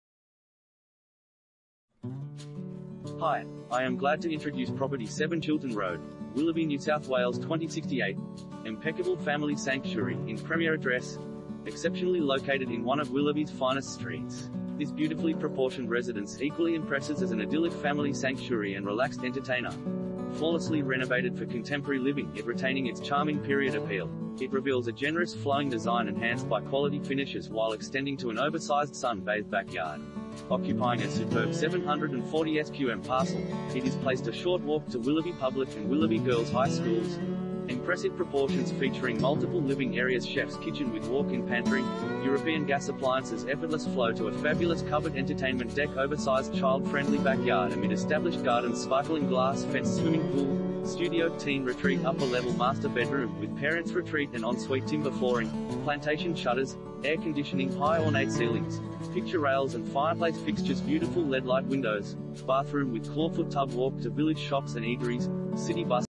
7 Chiltern Road, Willoughby NSW 2068 | AuPropertyReport.Com
7 Chiltern Road, Willoughby NSW 2068
Property Type: house
Bedrooms: 4
Bathrooms: 4
Car Space: 1
Impeccable family sanctuary in premier address